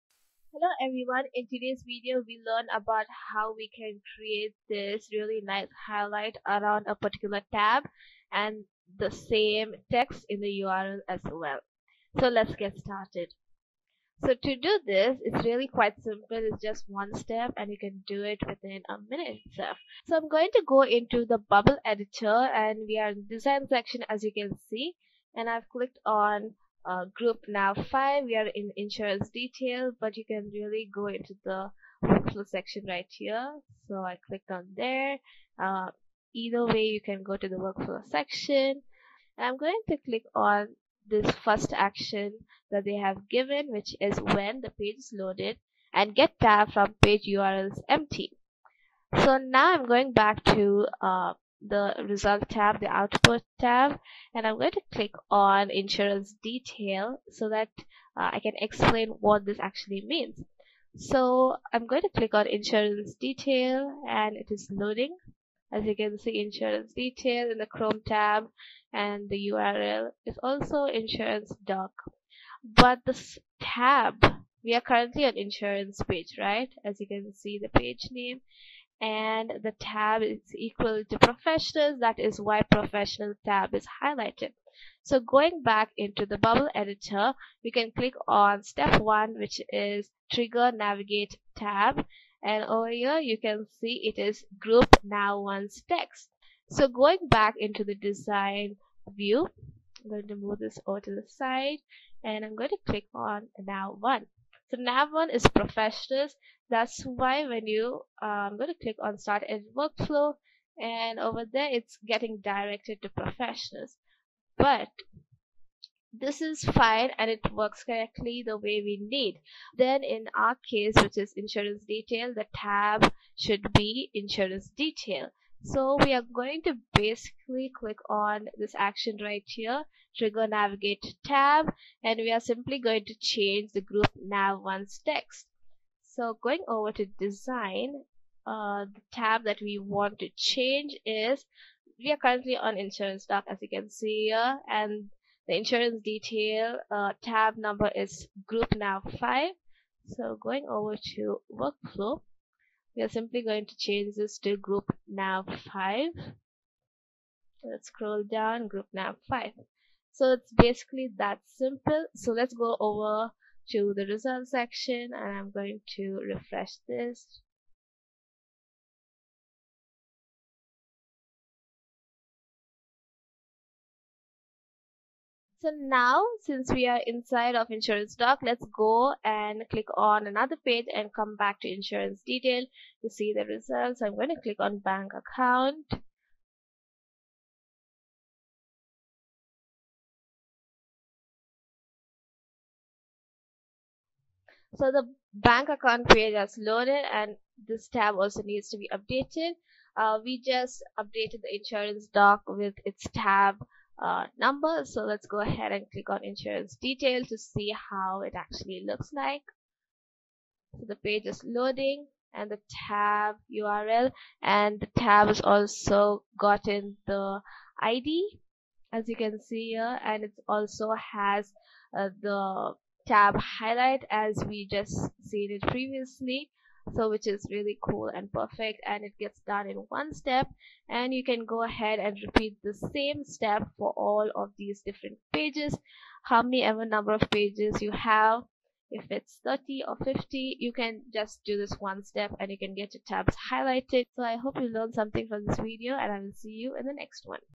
11 - How to get the Tab Highlight and URL - Bubble & Canvas Development
This video will show you how you can create a Bubble app with absolutely no code from start to finish. This app saves information to the database and allows you to edit the same when needed.

You will learn how to connect the nav tabs to the database and have a smooth workflow throughout the project.